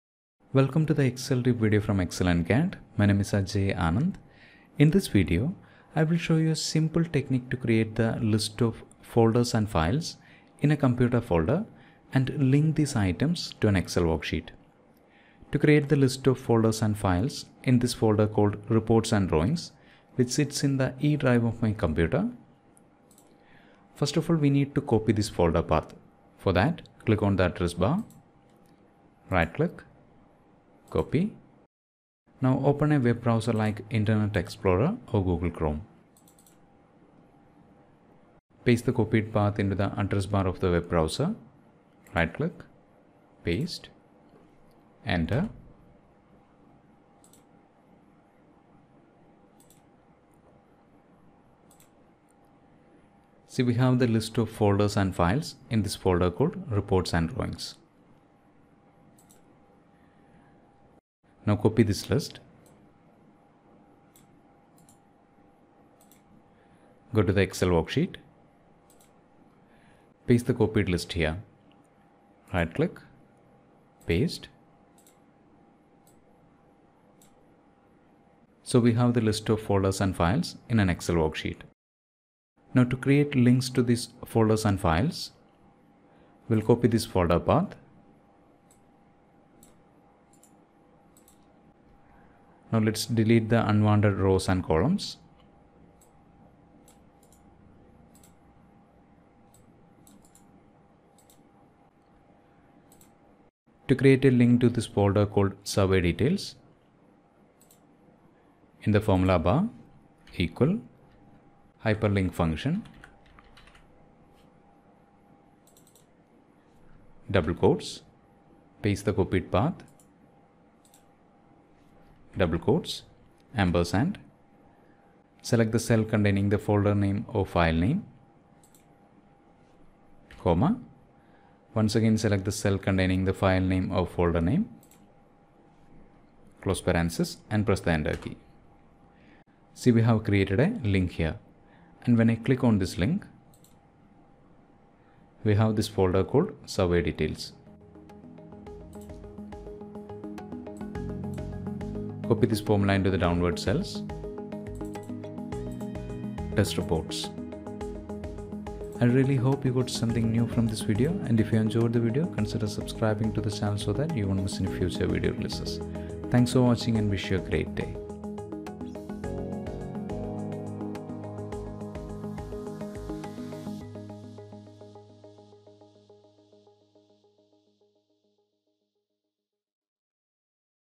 Create the list of files in a folder and link it to Excel [without VBA]
A simple method to create a list of files and folders and link it to an Excel Worksheet without VBA is explained in this video.

📊 Think Like an Analyst: Books That Helped Me
📚 Mind-Opening Reads You Can’t Miss